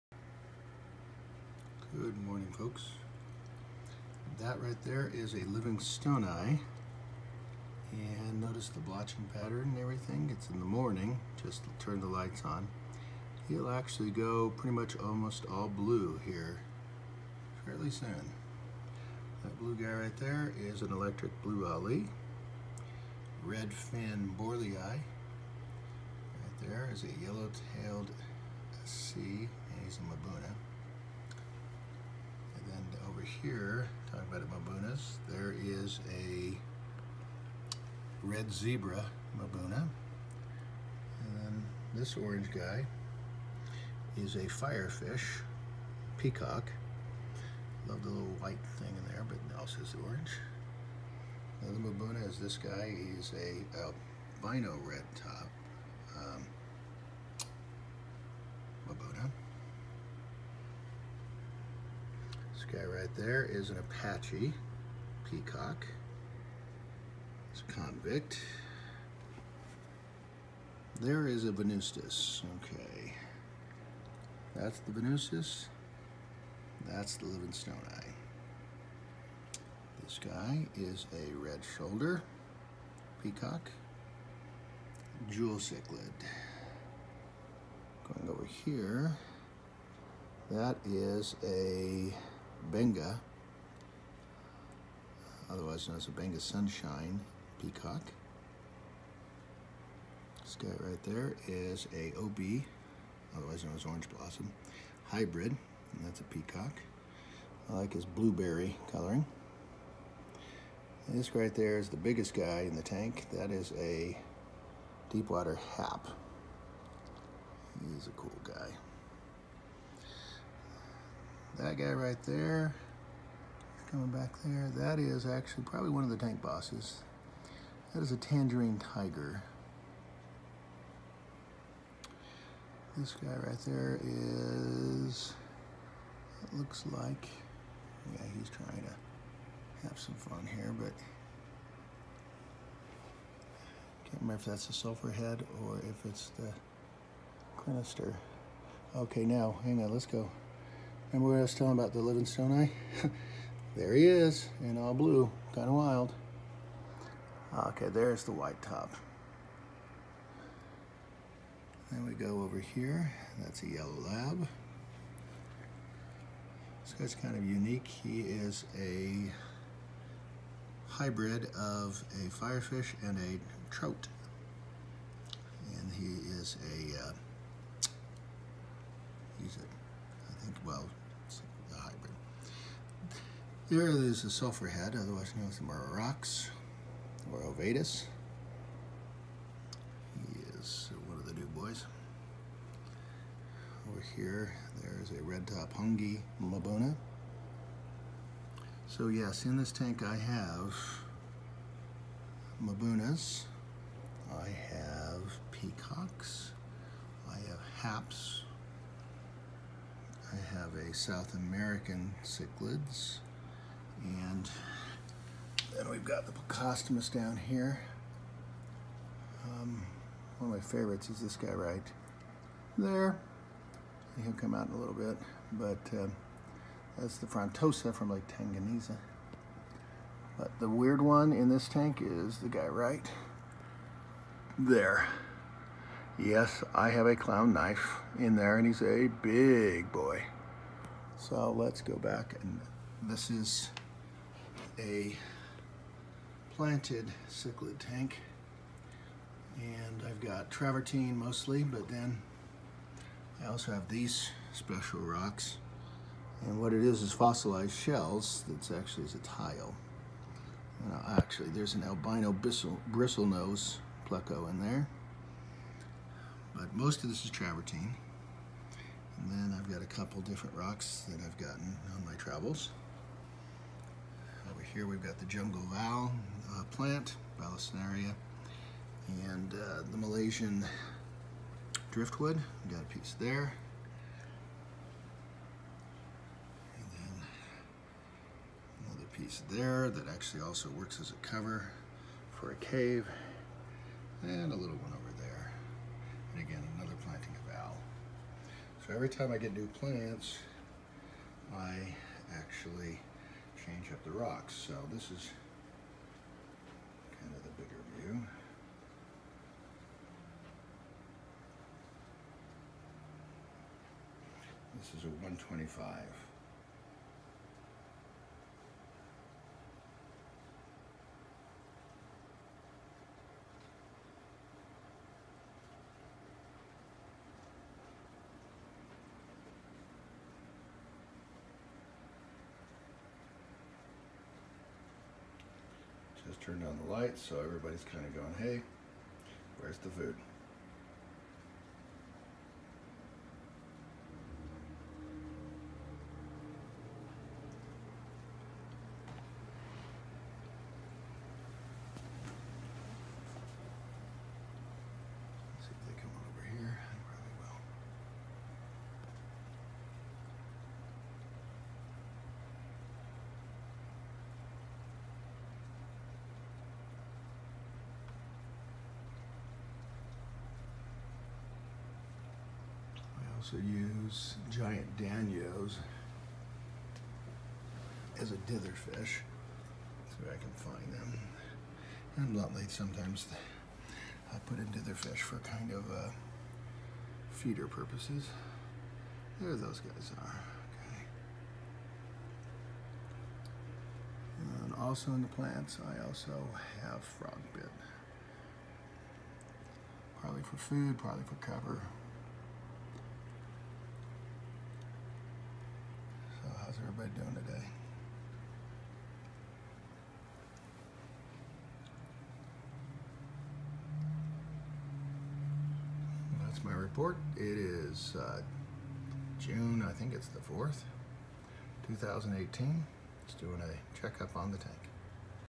My 125 Gallon Cichlid Aquarium Tour with fish names June 4, 2018
My 125 Gallon Cichlid Aquarium Tour with fish names June 4, 2018 . New round of cichlids. Moved rocks, plants and Malaysian wood to reset territories.